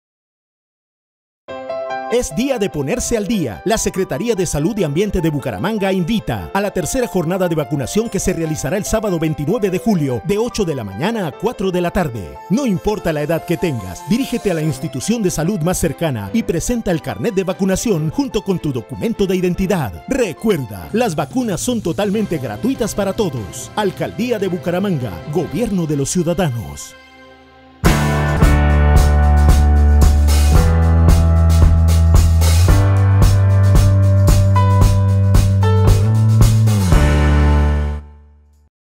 ¡Es hora de ponerte al Día, Tercera jornada de Vacunación!
Es hora ponerte al Día, te esperamos en la Tercera Jornada de vacunación este Sábado 29 en cualquier instituto de salud totalmente gratis!